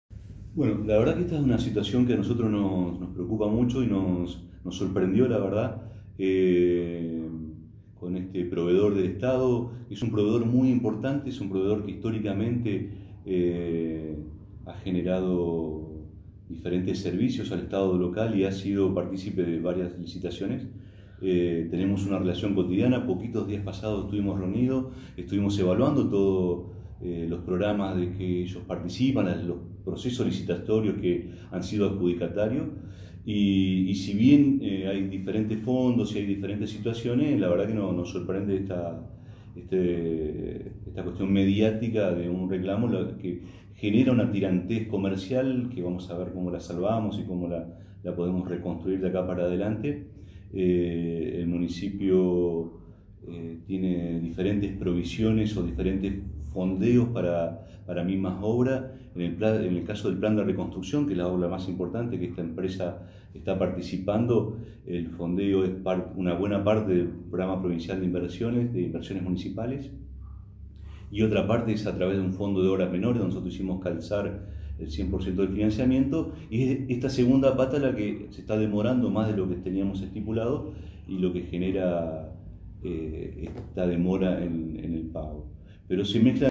La Municipalidad hace su descargo sobre la deuda que reclama la empresa Favial
Lo hizo a través de dos videos, donde Leopoldo Bauducco Secretario de Obras Públicas, realiza una exposición, en la cual deja claro la postura del DEM, con respecto a la nota enviada por el titular de la empresa Favial Eduardo Faudone, en la cual solicita al cuerpo legislativo, que instrumente las herramientas necesarias, para que el ejecutivo le otorgue una audiencia y así poder encontrar una solución a la abultada deuda que la municipalidad mantiene con su empresa.